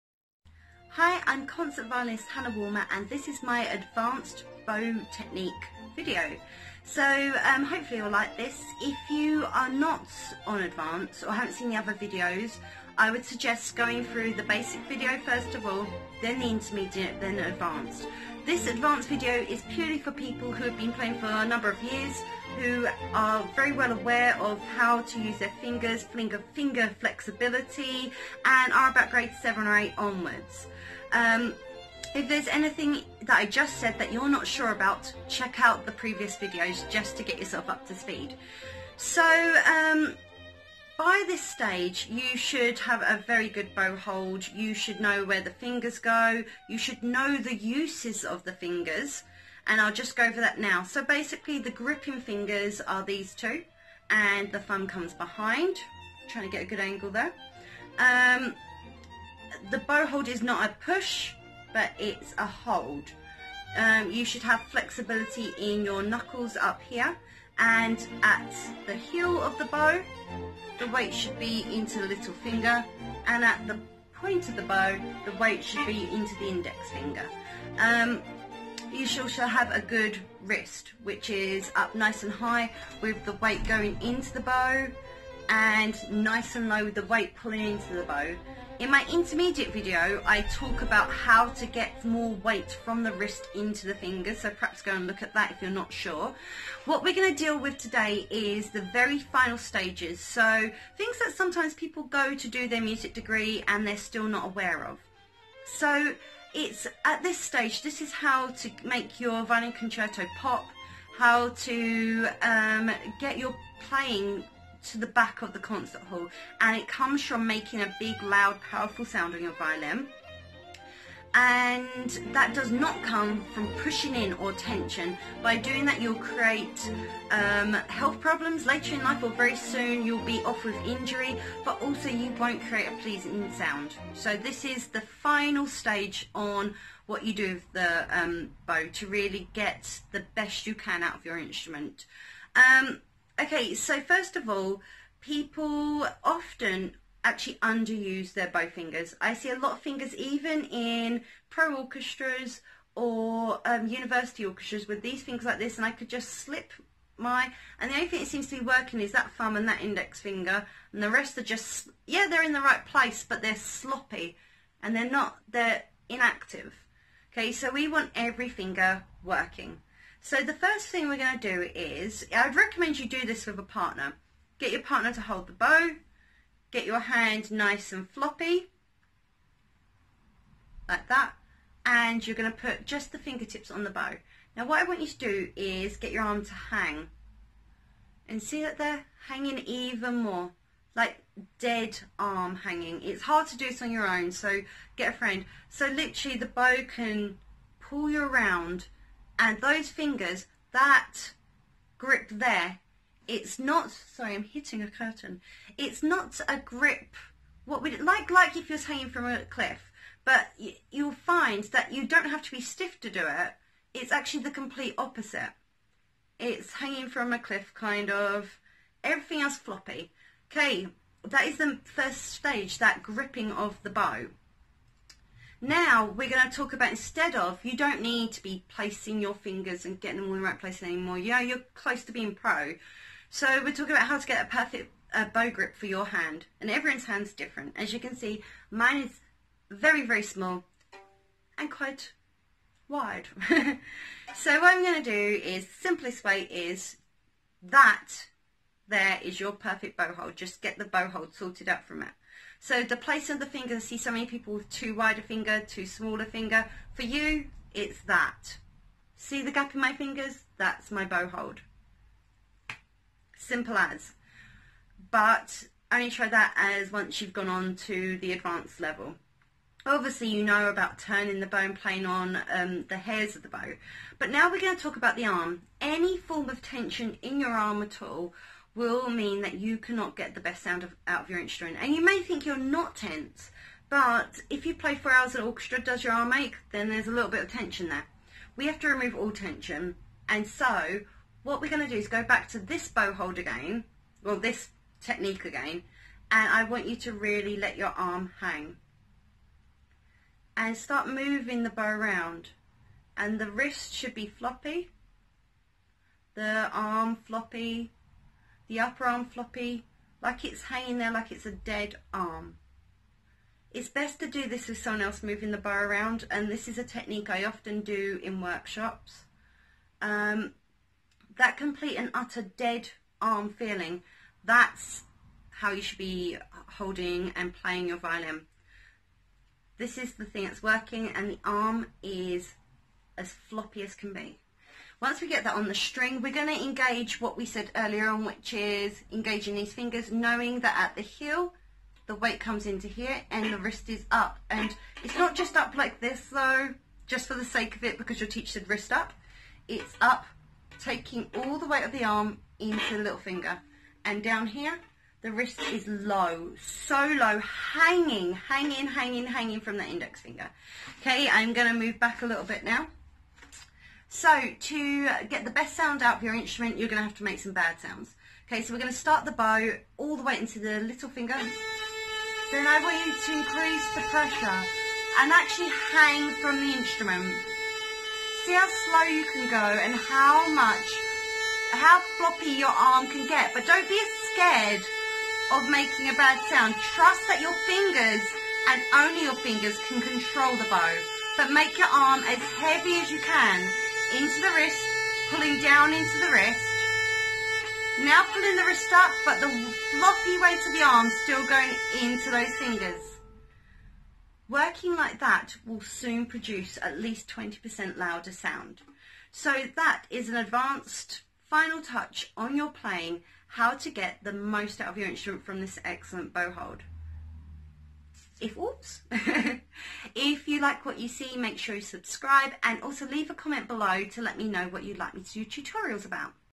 Violin Bow Hold - Advanced
Concert violinist Hannah Woolmer gives a short tutorial on bow hold for advanced players.  For beginner and intermediate work on bow hold checkout her previous tutorials.

All social - Hannah Woolmer